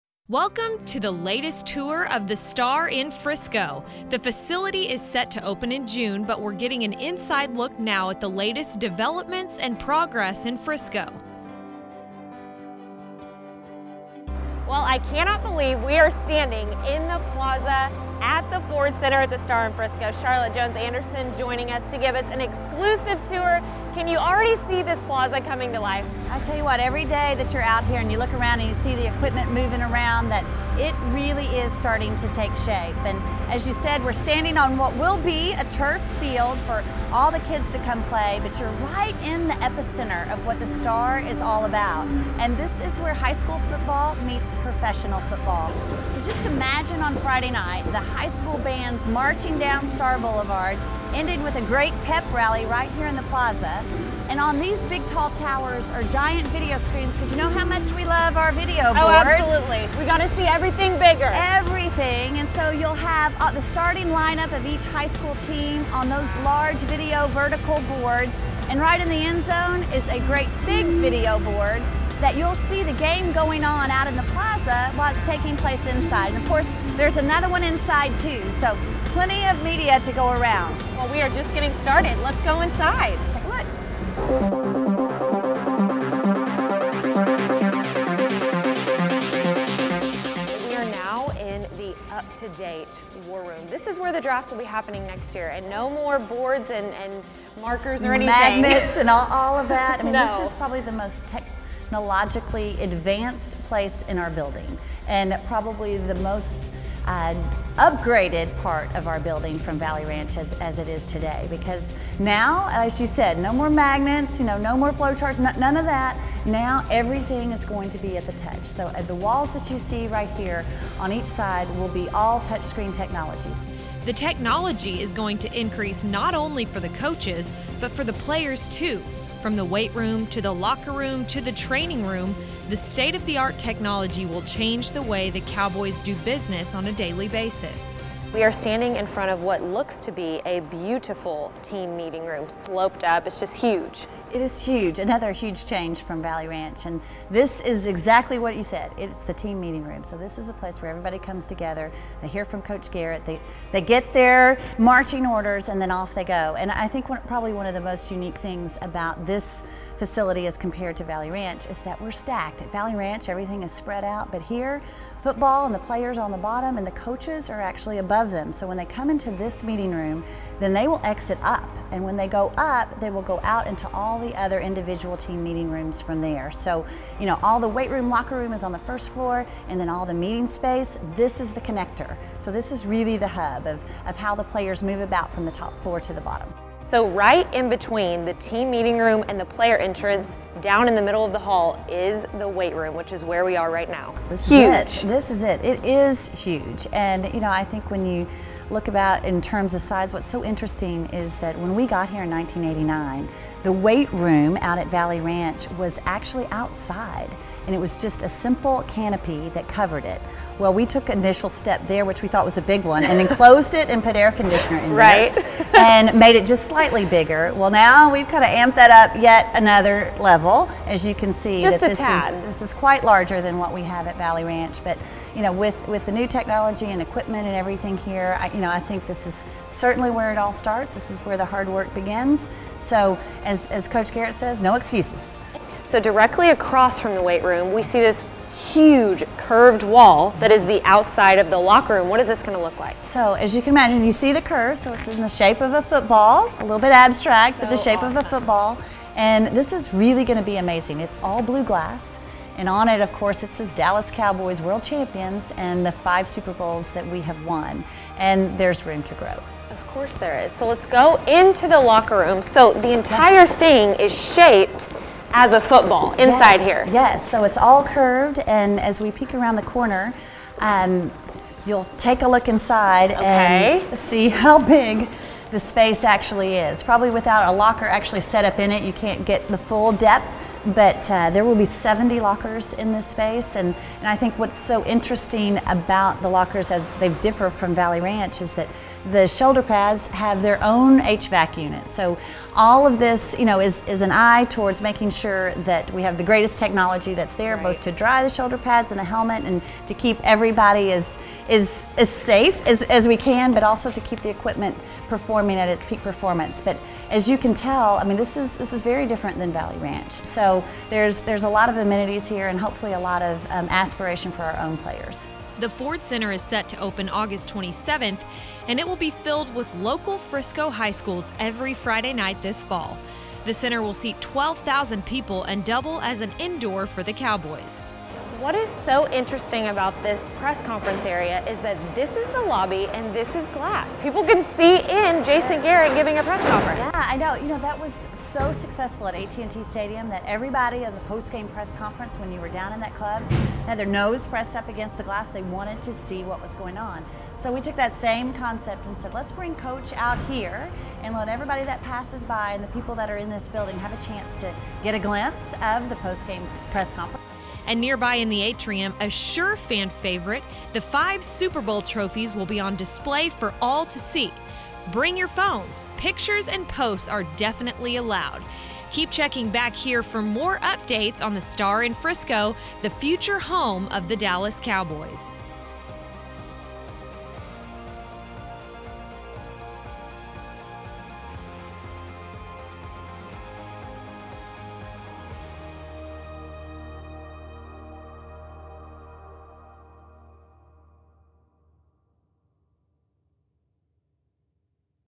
Exclusive Tour of The Star Construction with Charlotte Jones Anderson
Lindsay Draper of Dallas Cowboys TV takes a tour of the construction site with Charlotte Jones Anderson at The Star, the future home of the Dallas Cowboys.